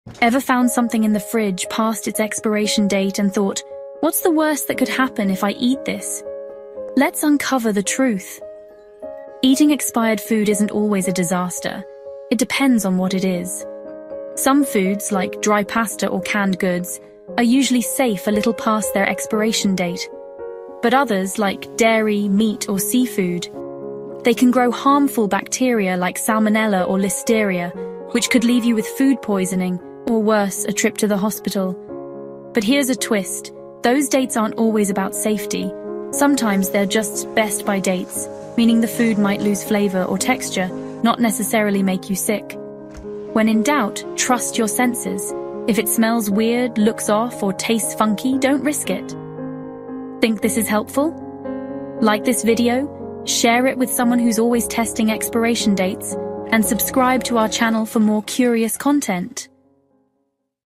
What Happens If You Eat Expired Food?
What happens when you eat expired food? Spoiled or safe? Watch this to know how your body reacts!